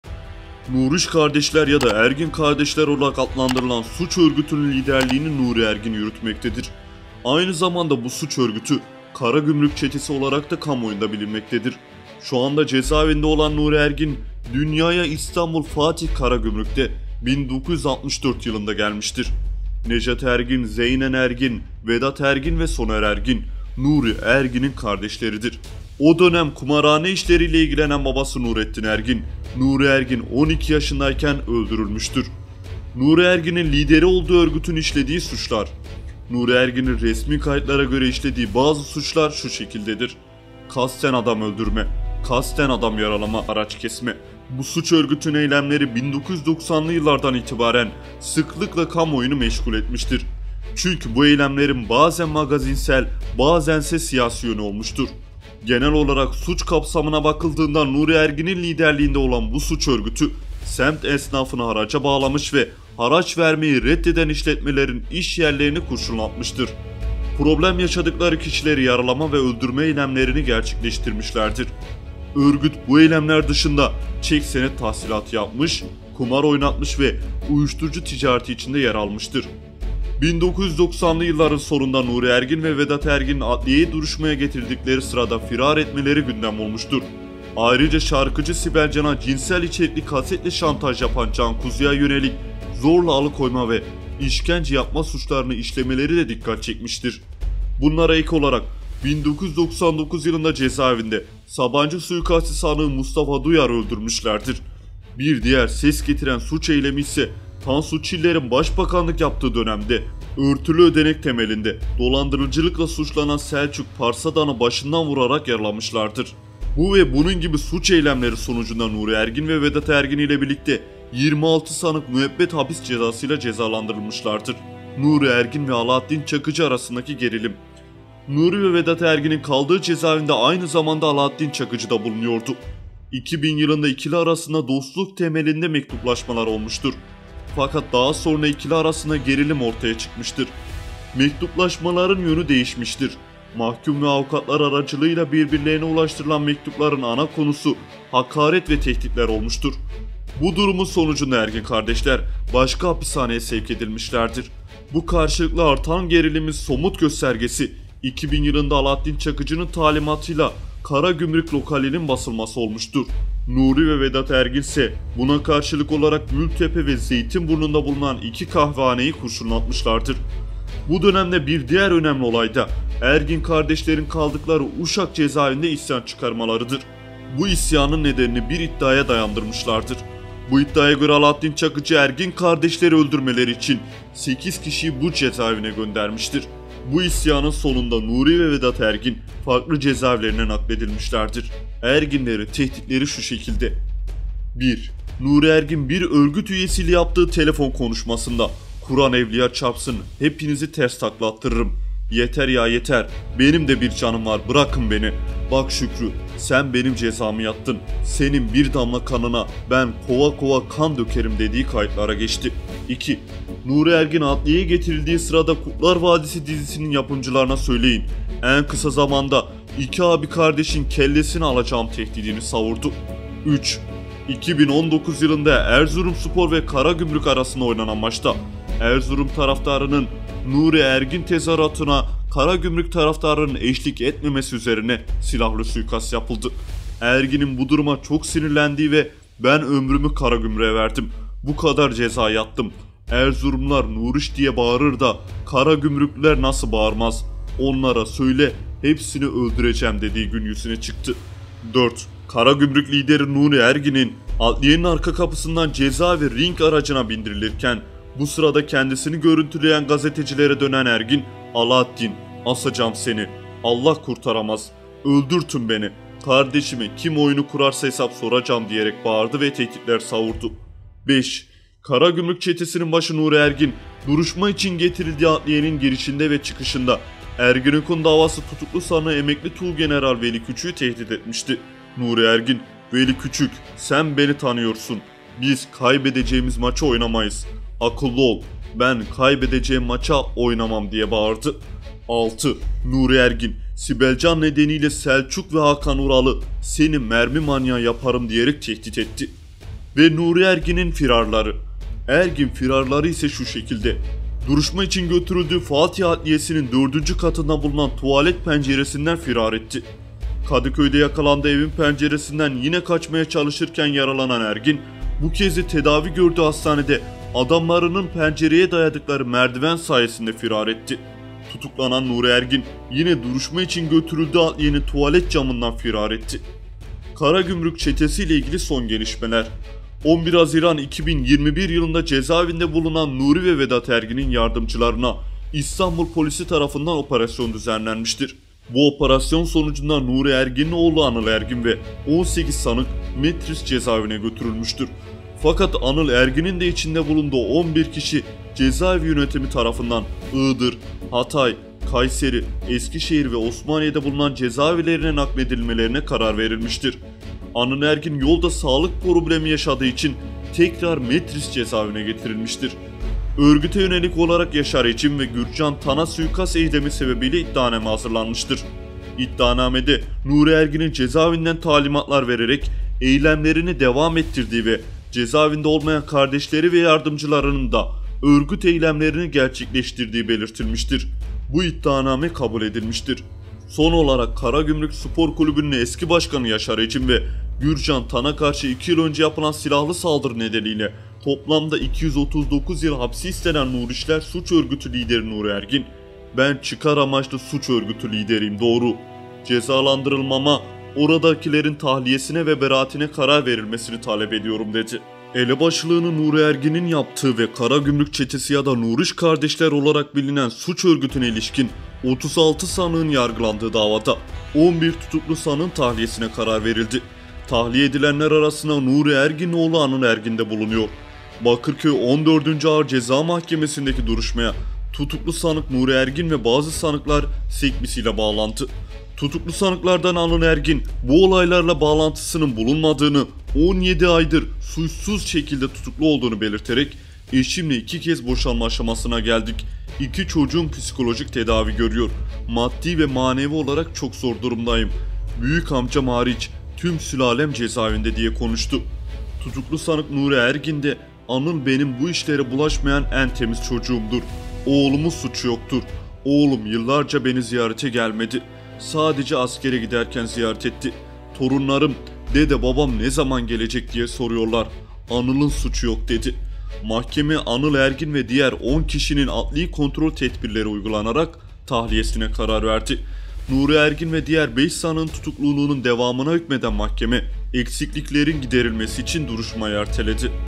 KARAGÜMRÜK ÇETESİ: NURİ ERGİN!
Karagümrük Çetesi veya Nuriş Kardeşler olarak bilinen Nuri ve Vedat Ergin kardeşler kimdir? Alaattin Çakıcı, Kurtlar Vadisi, Veli Küçük ve Uşak Cezaevi isyanı ile ilgili raconları ile gündemde olmuşlardır.